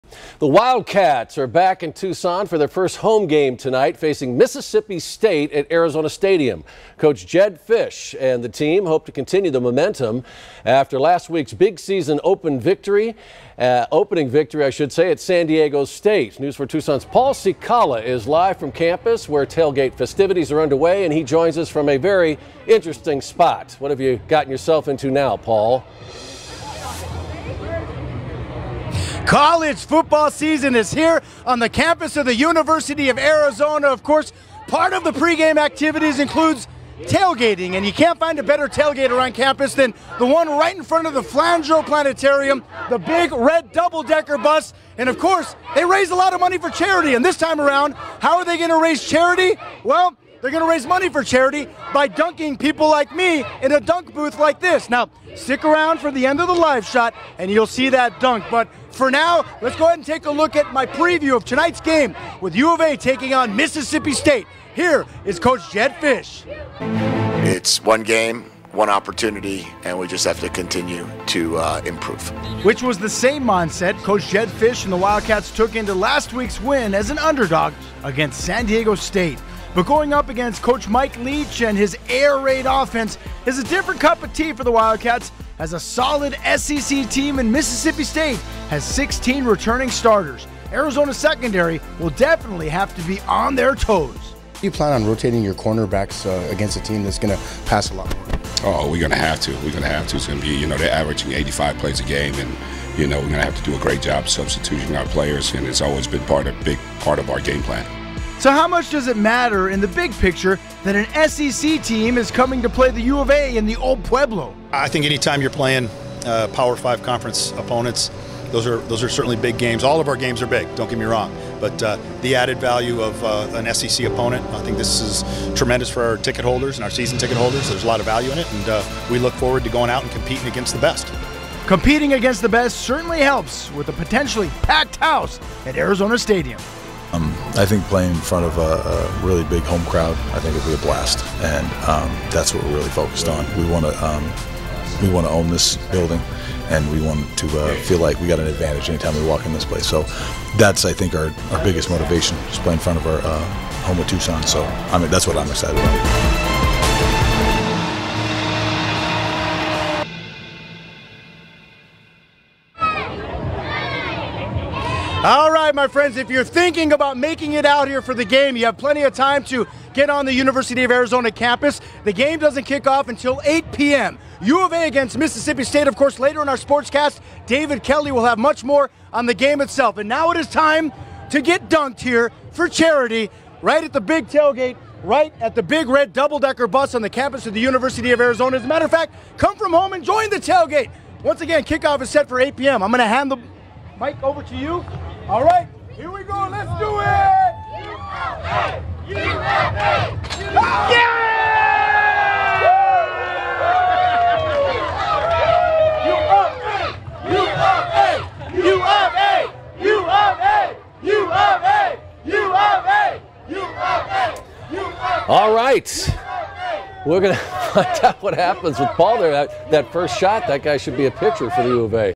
News 4 Tucson LIVE from UA Mall for Wildcats home opener
News 4 Tucson's Paul Cicala gets you set for the big game with Mississippi State while sitting on top of a dunk tank.